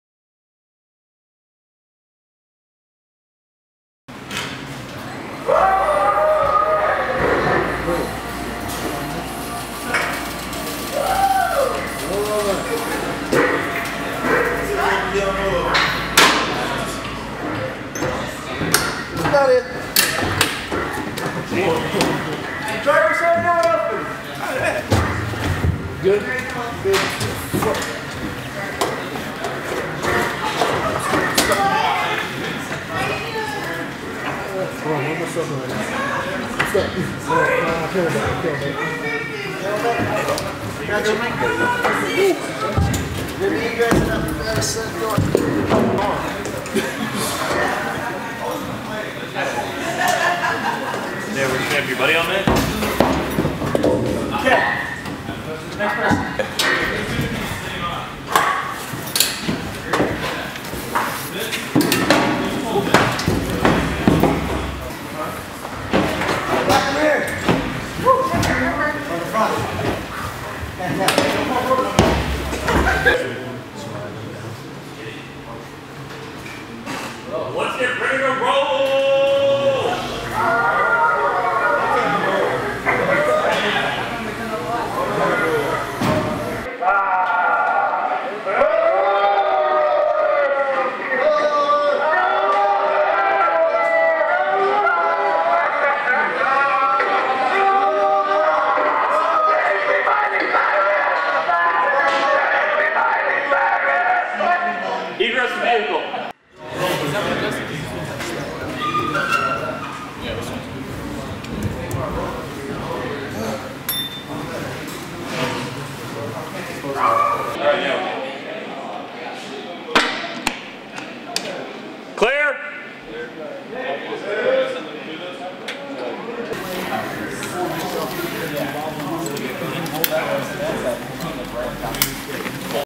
MilitaryTrainingCamps.com - MRAP Egress Training
- U.S. Army Soldiers, assigned to 412th Aviation Support Battalion, practice safe exit at the Training Support Activity Europe's (TSAE) Mine Resistant Ambush Protected (MRAP) Egress Trainer (MET), located at the 7th U.S. Army Joint Multinational Training Command's (JMTC) Grafenwoehr Training Area, Germany, Dec. 06, 2013. JMTC offers multiple training systems, facilities and resources for U.S. soldiers and partner nations forces. Also available in high definition. (U.S. Army video by Visual Information Specialist Markus Rauchenberger/released)